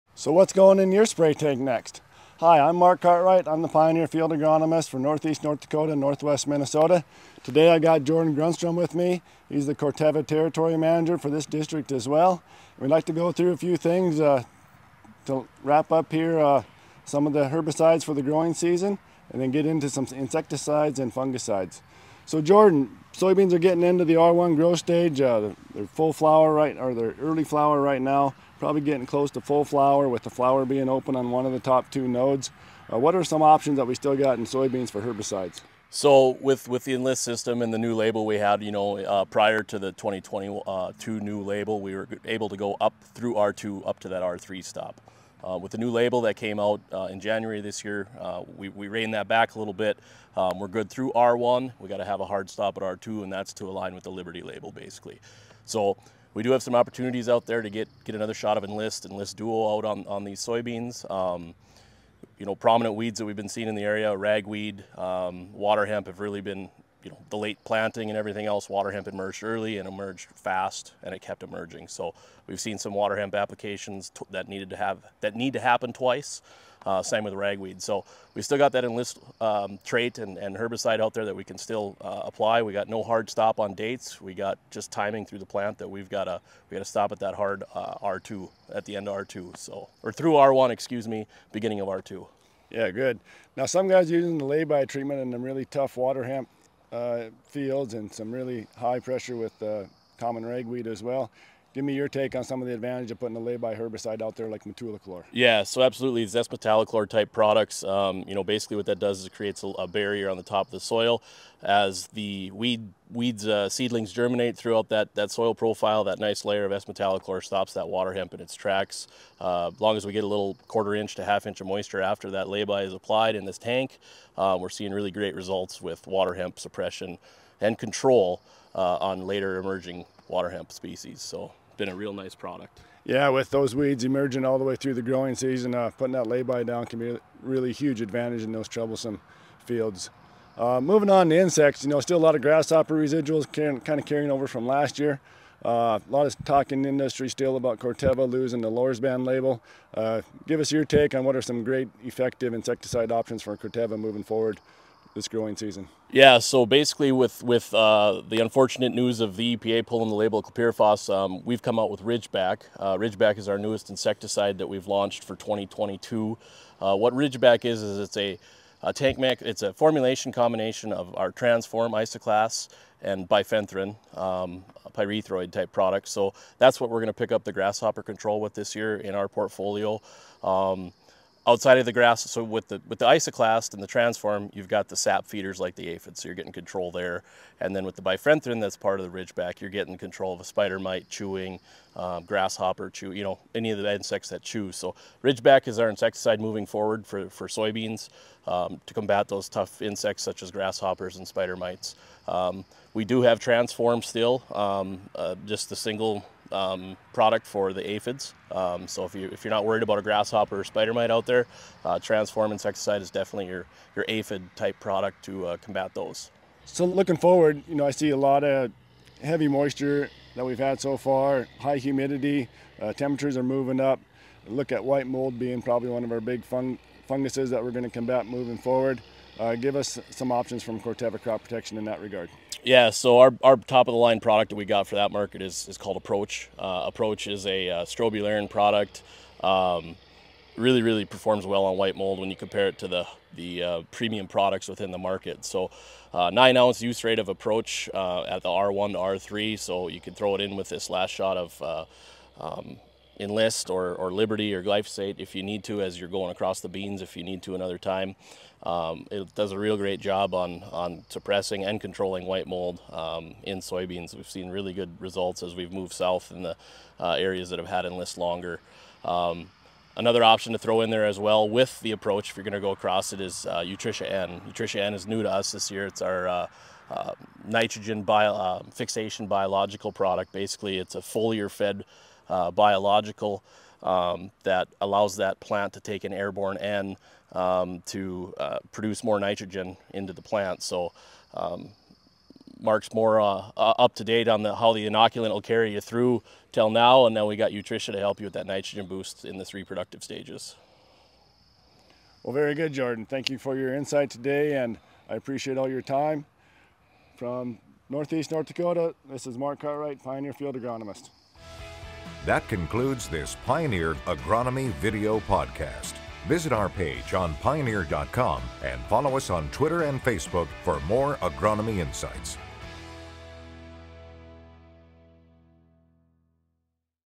Pioneer Agronomy Insights: Soybean Pests
Marc Cartwright, Pioneer Field Agronomist, and Jordan Grundstrom, Crop Protection Territory Manager, discuss pesticide, insecticide and fungicide options for soybean growers. Enlist herbicides can be applied to Pioneer brand Enlist E3 soybeans postemergence through the R1 growth stage. A wide application window provides more opportunities for better weed control. Aproach® Prima Fungicide is a broad-spectrum fungicide for control of foliar plant diseases and has preventive, curative, and systemic activity. To give plants an extra boost during the growing season, Utrisha™ N is an alternative nitrogen source that can supply actively growing plants with additional nitrogen to facilitate plant growth.

For more information on maximizing the benefits of weed control,